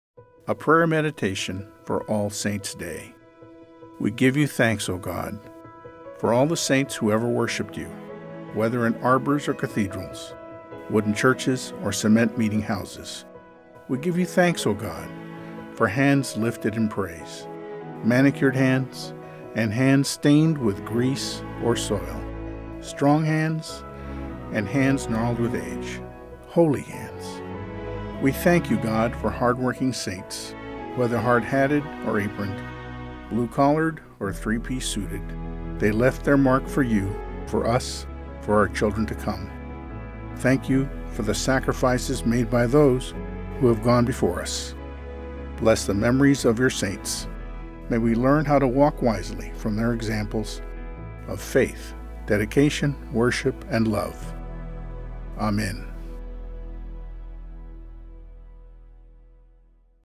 United Methodist Meditation for All Saints Day
All Saints Day, November 1, is a time to remember Christians of every time and place, honoring those who lived faithfully and shared their faith with us. Adapted from a prayer written by Safiyah Fosua, this video reflection can be used to remember those past and present whose examples inspire your own faith journey.